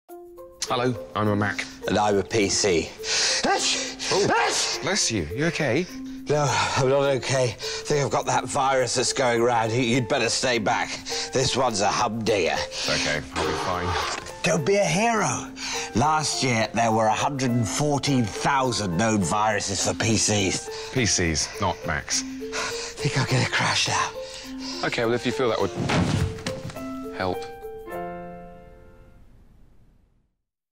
Apple - UK "Get A Mac" campaign - "Viruses" - 2006
The U.K. version of "Viruses" from the "Get A Mac" series